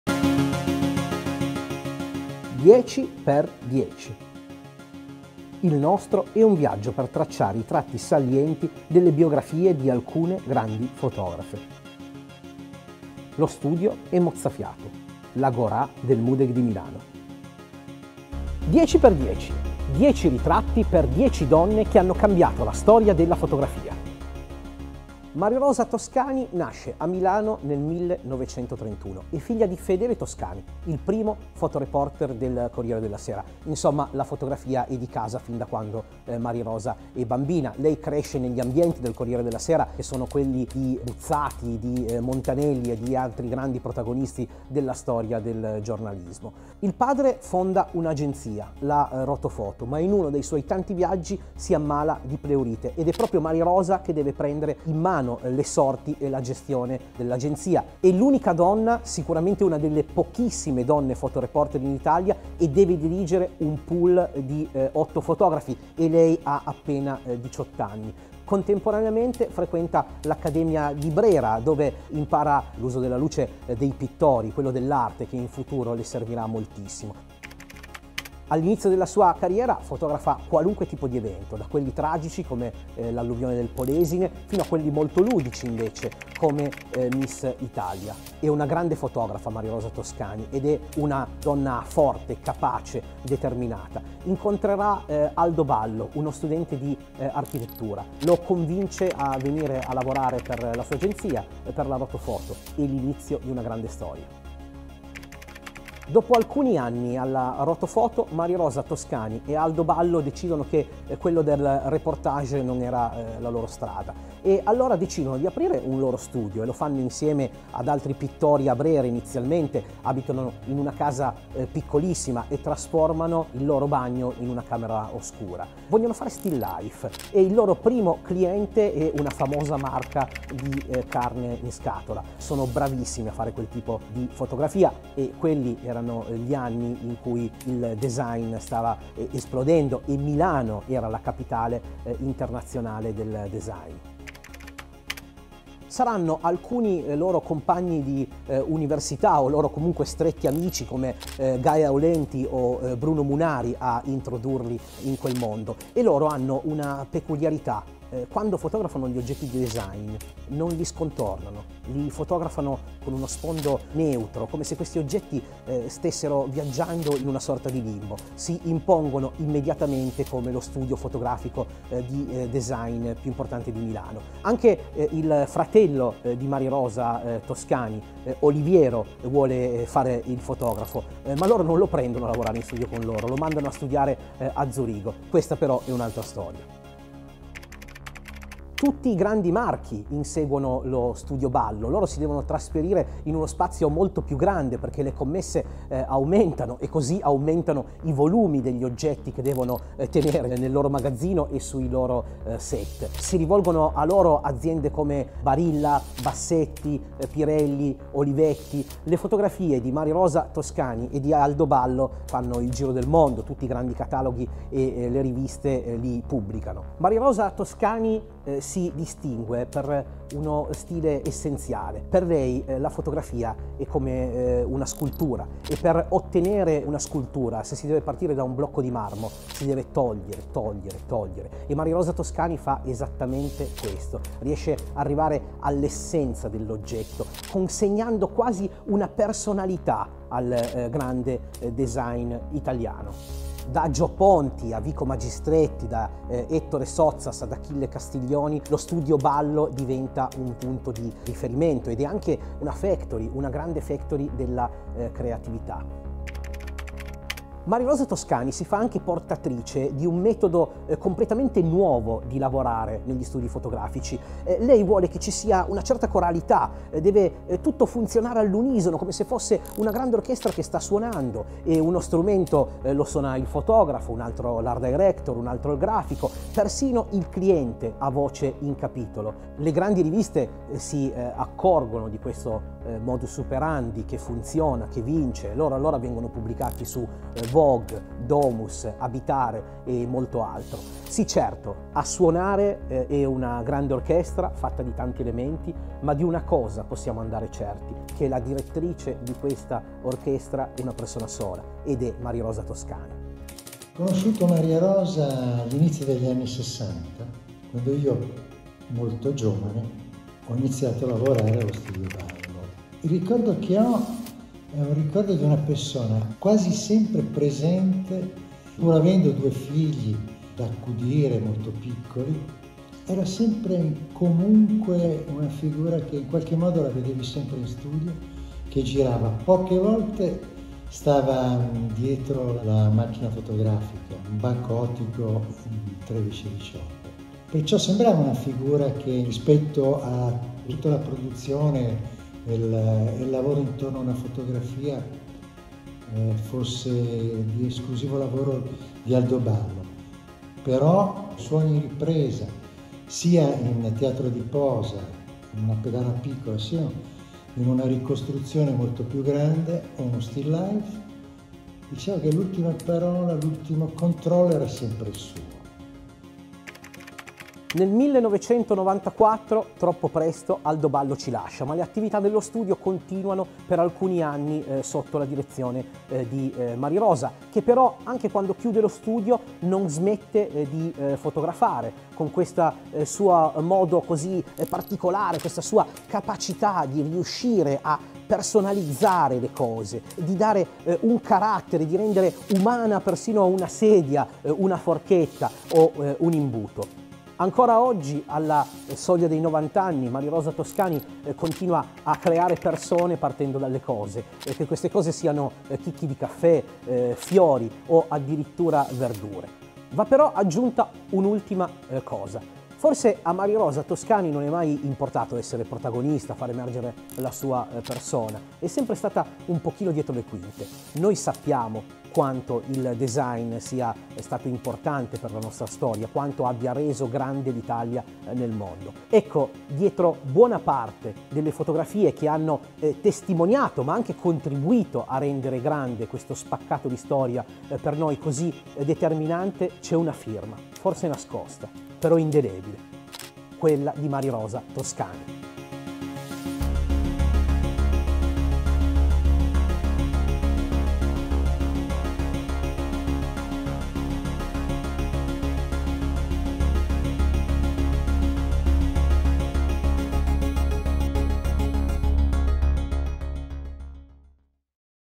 dieciXdieci10 ritratti per 10 donne che hanno cambiato la storia della fotografia.
Nella quinta puntata di 10x10, Nicolas Ballario ci accompagna alla scoperta della grande fotografa di design Marirosa Toscani Ballo.

Marirosa Toscani nasce a Milano nel 1931.
Nel 1951 incontra Aldo Ballo, uno studente al terzo anno di architettura. Insieme danno vita, a Milano, allo Studio fotografico Ballo.

Da Gio Ponti a Vico Magistretti, da Ettore Sottsass ad Achille Castiglioni: lo Studio Ballo diventa un punto di riferimento per i designer nel periodo più affascinante e vivace del design mondiale. Le loro fotografie costituiscono, fin da subito, l’anima del design italiano.

Marirosa Toscani Ballo si distingue per lo stile essenziale, le sue fotografie mettono in luce la bellezza e la personalità degli oggetti.
Ancora oggi, alla soglia dei 90 anni, Marirosa Toscani Ballo continua a creare persone partendo dalle cose.